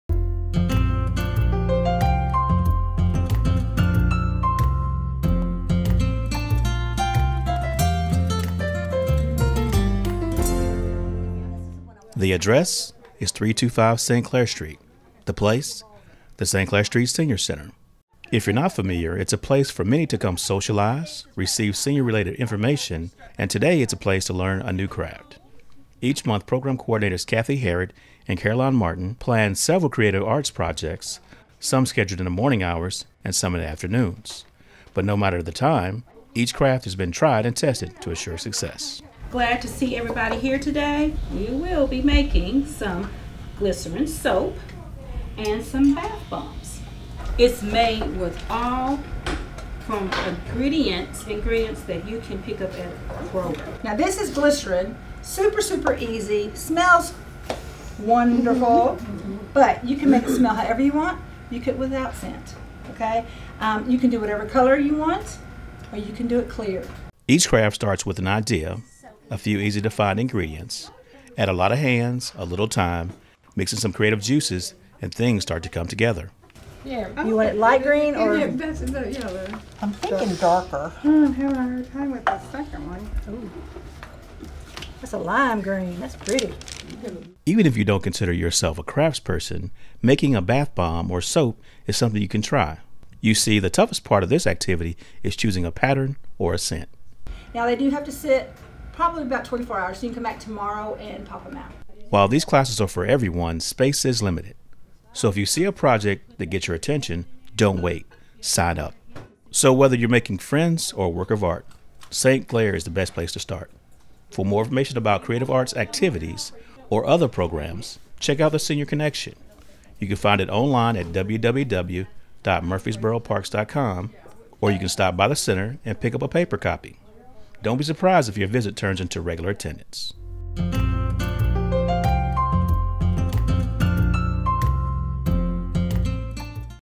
Creative Arts Class at St. Clair Senior Center
Learn to make bath bombs and soap, a craft you can share with family and friends.